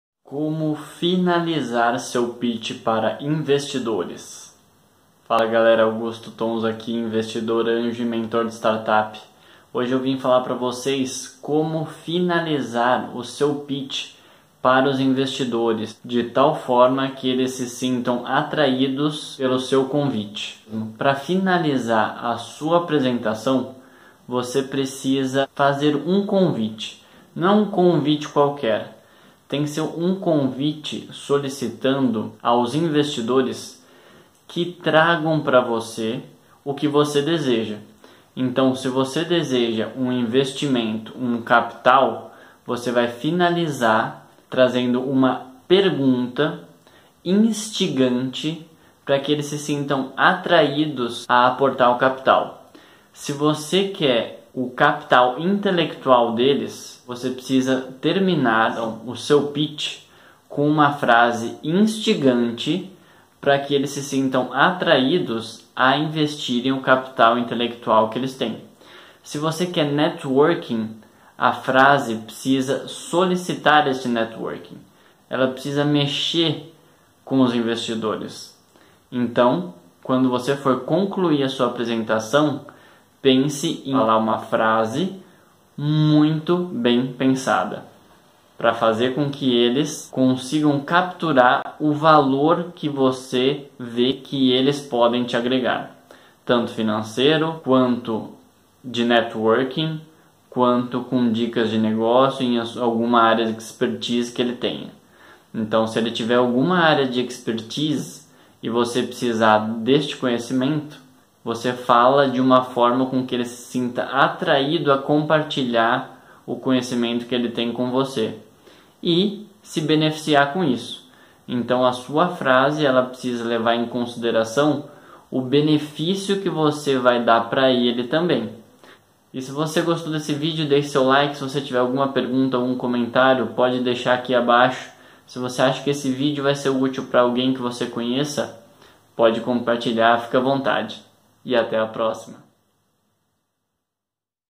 Como finalizar seu pitch para investidores?
A primeira impressão é muito importante, mas a última é a que vai ficar na memória do seu futuro investidor! Venha aprender a melhor forma de finalizar o seu pitch e aumentar suas chances de conseguir seu investimento.